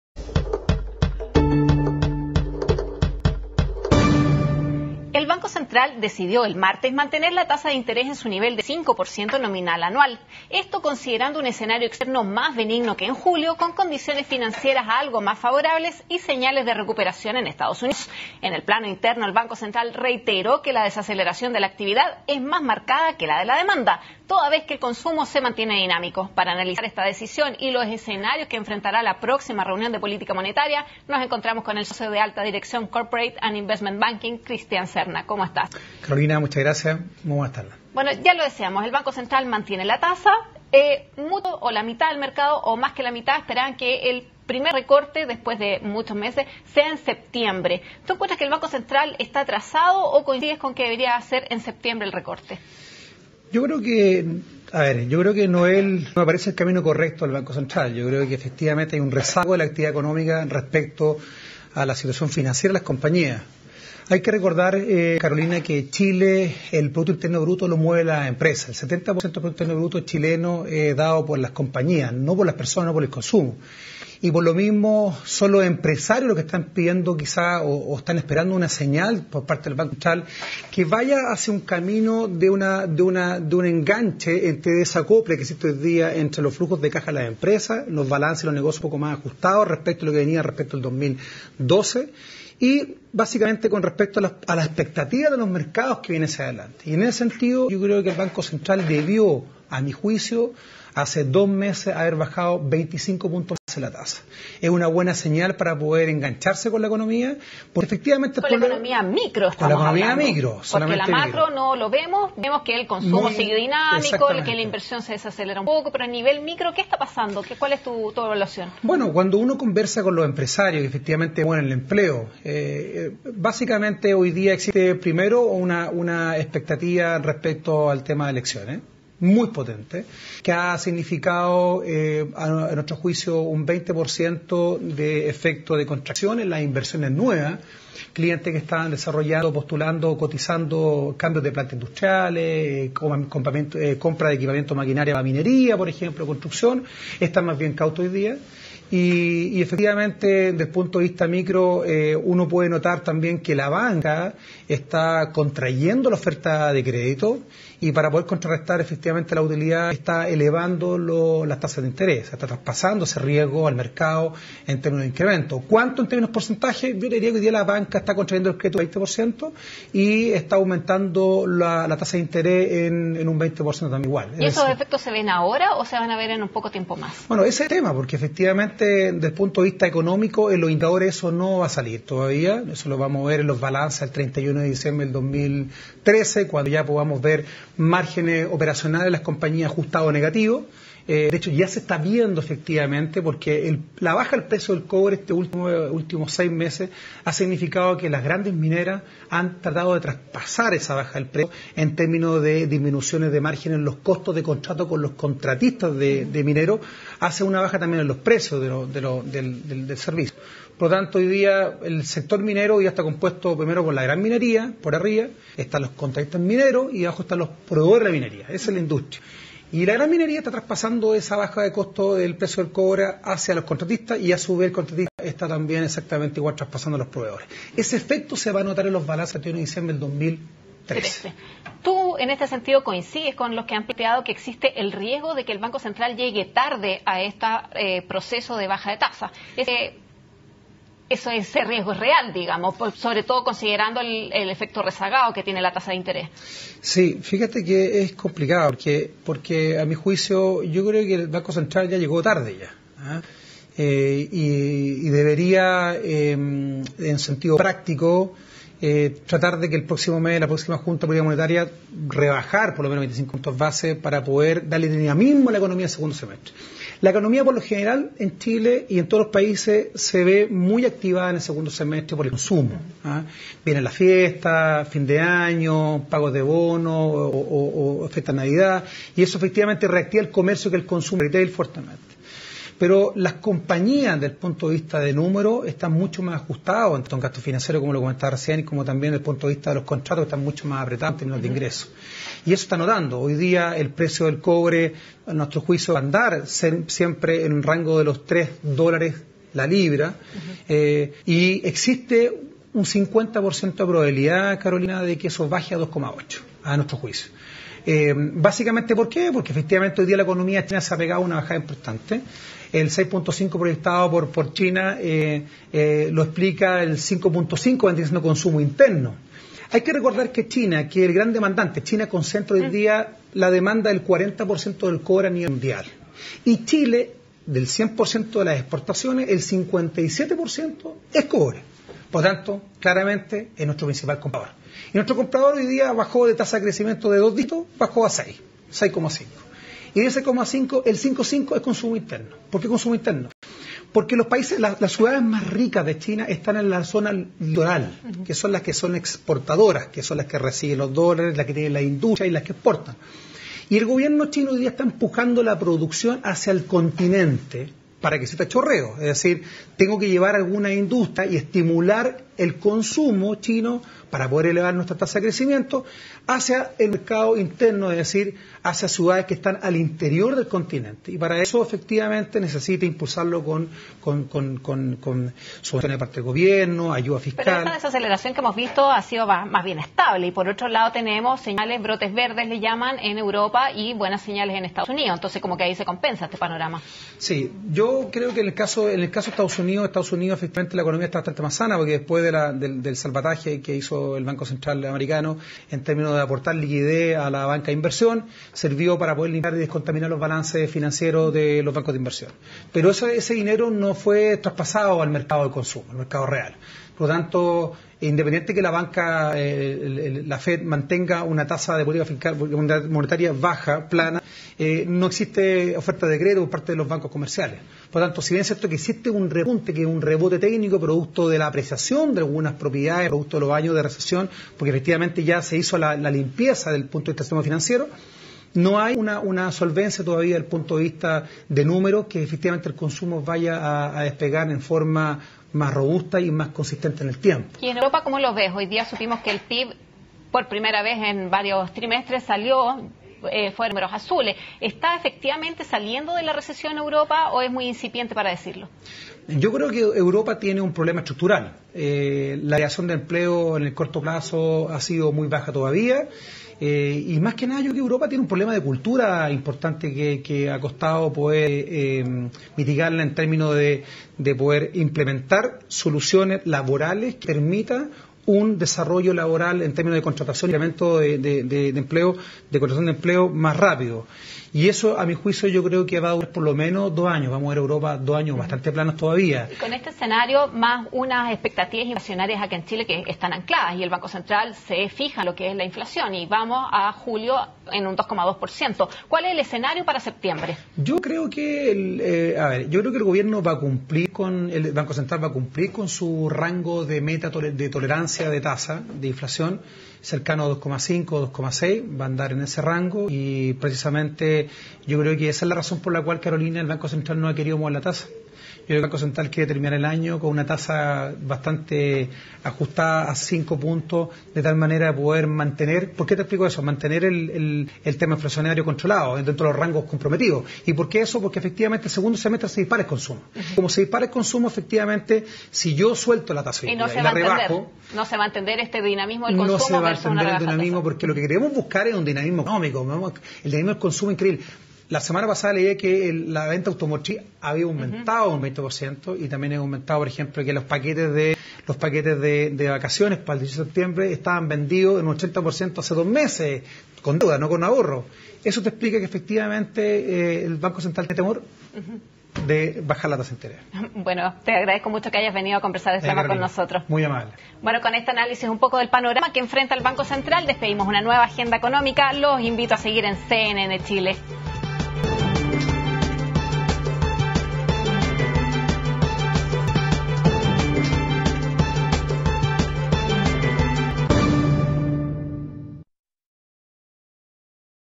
El escenario de la política monetaria en Chile
Este martes el Banco Central decidió mantener la tasa de interés en su nivel de 5% nominal anual. En el plano interno, el organismo reiteró que la desaceleración de la actividad es más marcada que la de la demanda, mientras que el consumo se mantiene dinámico. Acerca de esto conversó con Agenda Económica el socio de AltaDirección Corp & Investment Bancking, Cristián Cerna.